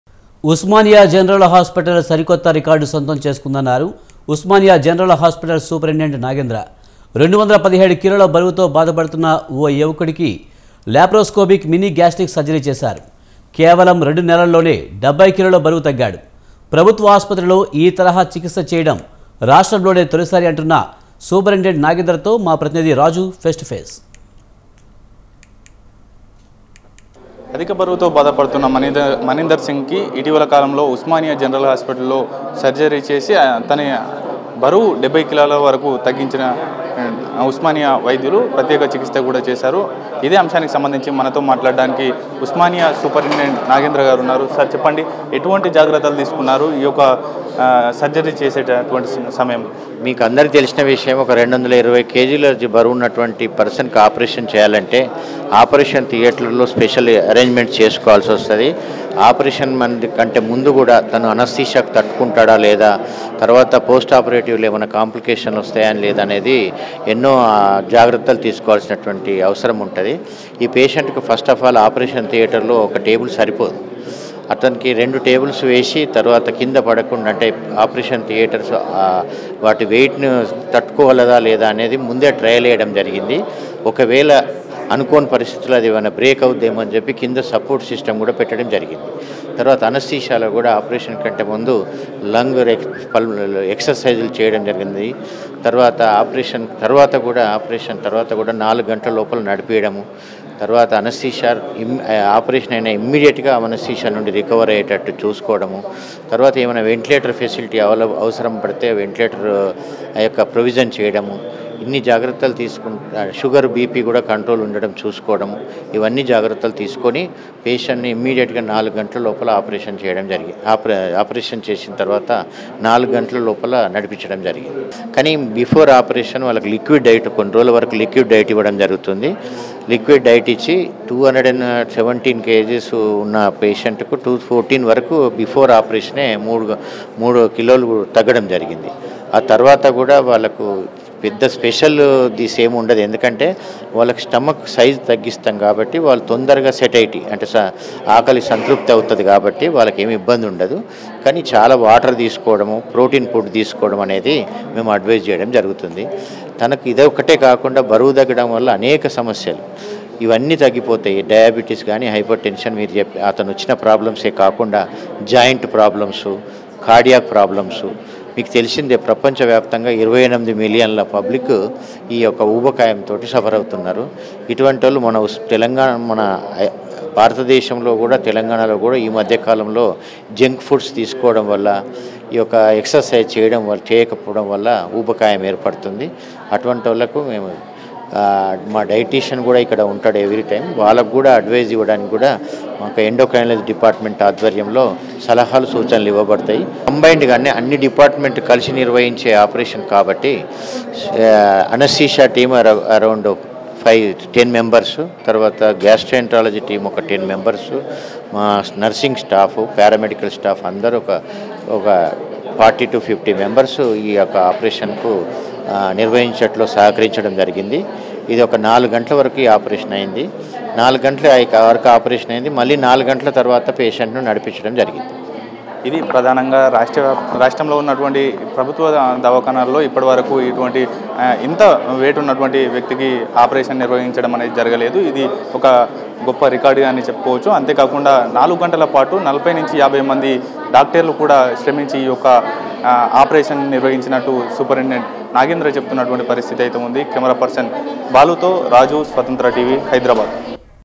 అధిక బరువున్న యువకుడికి ఆపరేషన్‌.. 70కిలోలు తగ్గింపు.!-డాక్టర్‌ నాగేంద్ర | Swatantra TV
Operation for an overweight young man.. 70 kg reduction.!-Dr. Nagendra | Swatantra TV

(ఉస్మానియా ఆసుపత్రిలో.. సరికొత్త రికార్డు..! )
ఎక్స్ క్లూజివ్ ప్రోగ్రామ్స్: